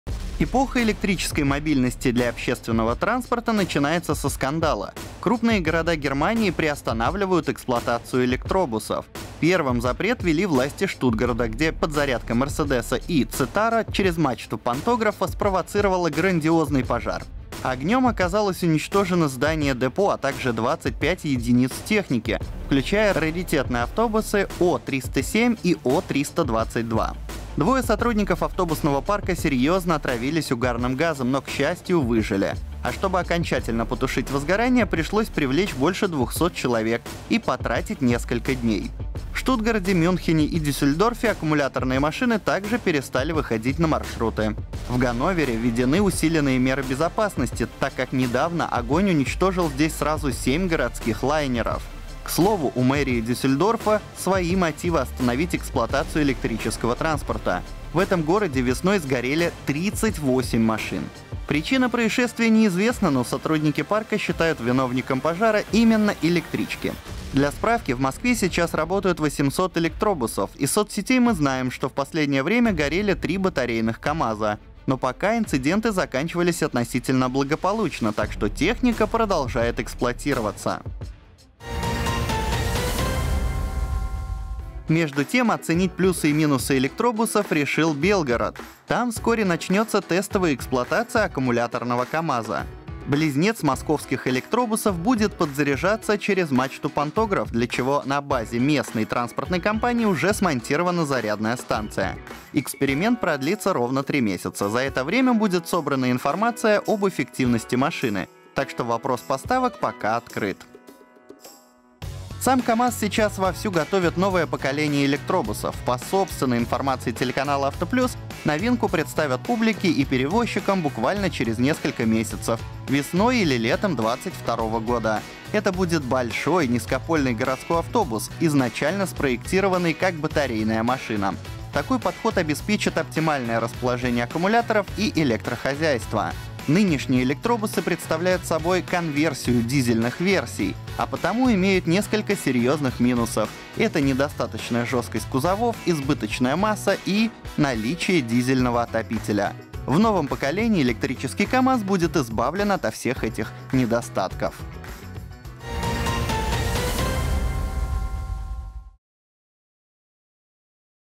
Германия останавливает эксплуатацию электробусов, а КамАЗ готовит новое их поколение | Новости 1735
Эпоха электрической мобильности для общественного транспорта начинается со скандала: крупные города Германии приостанавливают эксплуатацию электробусов. Первым запрет ввели власти Штутгарта, где подзарядка Мерседеса eCitaro через мачту пантографа спровоцировала грандиозный пожар. Огнём оказалось уничтожено здание депо, а также 25 единиц техники, включая раритетные автобусы O307 и O322. Двое сотрудников автобусного парка серьёзно отравились угарным газом, но, к счастью, выжили. А чтобы окончательно потушить возгорание, пришлось привлечь больше двухсот человек и потратить несколько дней… В Штутгарте, Мюнхене и Дюссельдорфе аккумуляторные машины также перестали выходить на маршруты… В Ганновере введены усиленные меры безопасности, так как недавно огонь уничтожил здесь сразу семь городских «лайнеров». К слову, у мэрии Дюссельдорфа — свои мотивы остановить эксплуатацию электрического транспорта: в этом городе весной сгорели 38 машин. Причина происшествия неизвестна, но сотрудники парка считают виновником пожара именно «электрички». Для справки, в Москве сейчас работают 800 электробусов.

Между тем, оценить плюсы и минусы электробусов решил Белгород: там вскоре начнётся тестовая эксплуатация аккумуляторного КамАЗа. Близнец московских электробусов будет подзаряжаться через мачту-пантограф, для чего на базе местной транспортной компании уже смонтирована зарядная станция. Эксперимент продлится ровно три месяца. За это время будет собрана информация об эффективности машины, так что вопрос поставок пока открыт.

Сам КамАЗ сейчас вовсю готовит новое поколение электробусов. По собственной информации телеканала «Авто Плюс», новинку представят публике и перевозчикам буквально через несколько месяцев: весной или летом 2022 года. Это будет большой низкопольный городской автобус, изначально спроектированный как батарейная машина. Такой подход обеспечит оптимальное расположение аккумуляторов и электрохозяйства. Нынешние электробусы представляют собой конверсию дизельных версий, а потому имеют несколько серьёзных минусов: это недостаточная жёсткость кузовов, избыточная масса и… наличие дизельного отопителя. В новом поколении электрический КамАЗ будет избавлен ото всех этих недостатков.